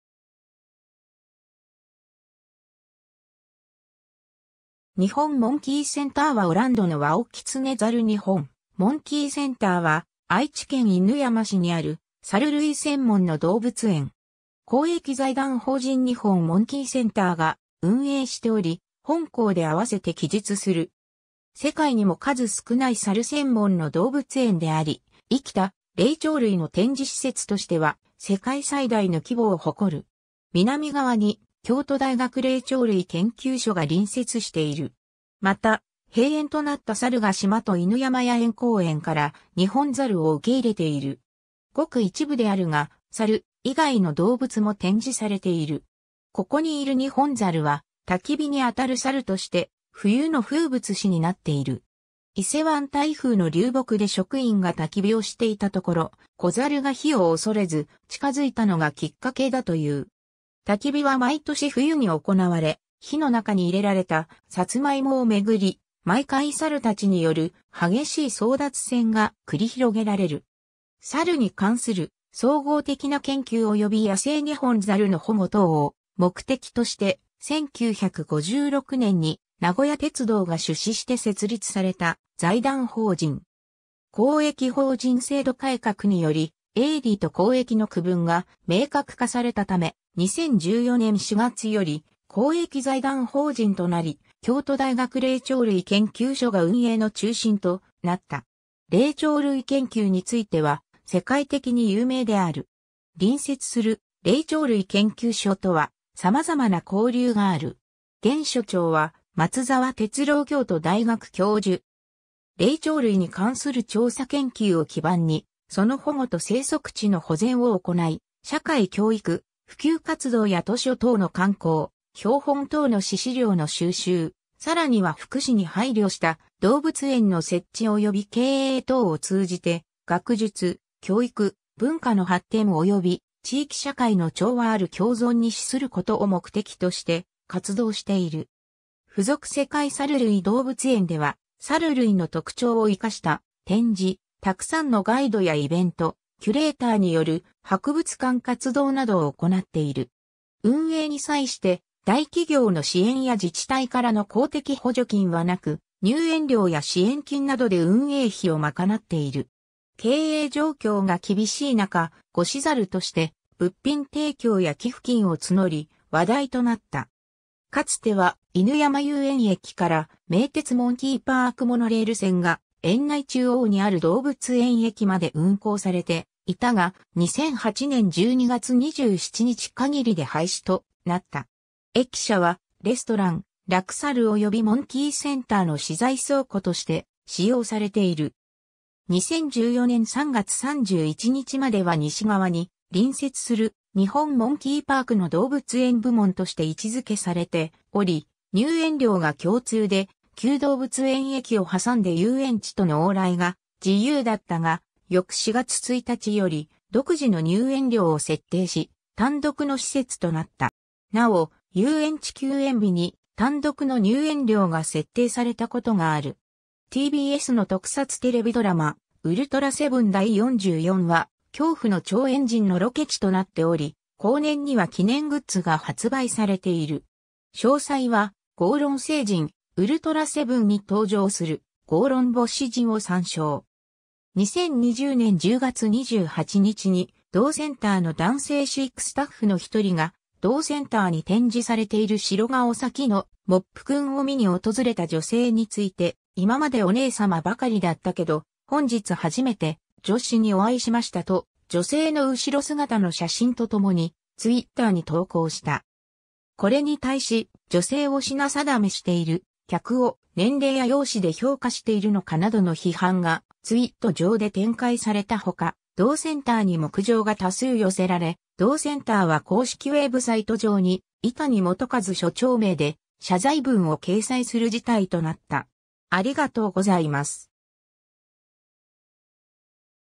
日本モンキーセンター
公益財団法人_(内閣総理大臣認定)
日本モンキーセンター Waoランドのワオキツネザル 日本モンキーセンター（にほんモンキーセンター）は、愛知県犬山市にある、サル類専門の動物園。
公益財団法人日本モンキーセンターが運営しており、本項であわせて記述する。
 世界にも数少ないサル専門の動物園であり、生きた霊長類の展示施設としては世界最大の規模を誇る。
南側に京都大学霊長類研究所が隣接している。
また、閉園となった猿ヶ島と犬山野猿公苑からニホンザルを受け入れている。
 ごく一部であるが、サル以外の動物も展示されている (KIDSZOO)。
 ここにいるニホンザルは、焚き火に当たるサルとして冬の風物詩になっている。
伊勢湾台風の流木で職員が焚き火をしていたところ、子ザルが火を恐れず近づいたのがきっかけだという。
焚き火は毎年冬に行われ、火の中に入れられたサツマイモをめぐり、毎回サルたちによる激しい争奪戦が繰り広げられる。
 サルに関する総合的な研究及び野生ニホンザルの保護等を目的として、1956年（昭和31年）に名古屋鉄道が出資して設立された財団法人。
公益法人制度改革により営利と公益の区分が明確化されたため、2014年4月より公益財団法人となり、京都大学霊長類研究所が運営の中心となった。
霊長類研究については世界的に有名である。
隣接する霊長類研究所とは、さまざまな交流が有る。
現所長は松沢哲郎京都大学教授。
 「霊長類に関する調査研究を基盤に、その保護と生息地の保全を行い、社会教育・普及活動や図書等の刊行、標本等の資試料の収集、さらには福祉に配慮した動物園の設置および経営等を通じて、学術・教育・文化の発展及び地域社会の調和ある共存に資すること（定款より）」を目的として活動している。
附属世界サル類動物園では、サル類の特徴を活かした展示、たくさんのガイドやイベント、キュレーターによる博物館活動などを行っている。
 運営に際して、大企業の支援や自治体からの公的補助金はなく、入園料や支援金などで運営費を賄っている。
経営状況が厳しい中、「ご支猿」として物品提供や寄付金を募り、話題となった。
 かつては、犬山遊園駅から名鉄モンキーパークモノレール線が園内中央にある動物園駅まで運行されていたが、2008年12月27日限りで廃止となった。
駅舎はレストラン「楽猿」及びモンキーセンターの資材倉庫として使用されている。
 2014年3月31日までは西側に隣接する「日本モンキーパーク」の動物園部門として位置付けされており、入園料が共通で旧動物園駅（モノレール・廃止）を挟んで遊園地との往来が自由だったが、翌4月1日より独自の入園料を設定し、単独の施設となった。
なお、遊園地休園日に単独の入園料が設定されたことがある。
 TBSの特撮テレビドラマ『ウルトラセブン』第44話「恐怖の超猿人」（1968年8月4日放送）のロケ地となっており、後年には記念グッズが発売されている。
詳細はゴーロン星人#『ウルトラセブン』に登場するゴーロン星人を参照。
 2020年10月28日に、同センターの男性飼育スタッフの一人が、同センターに展示されているシロガオサキの「モップくん」を見に訪れた女性について、「今までお姉様ばかりだったけど、本日初めて『女子』にお会いしました」と、女性の後ろ姿の写真と共にTwitterに投稿した。
これに対し「女性を品定めしている」「客を年齢や容姿で評価しているのか」などの批判がTwitte上で展開されたほか、同センターにも苦情が多数寄せられ、同センターは公式ウェブサイト上に伊谷原一所長名で謝罪文を掲載する事態となった。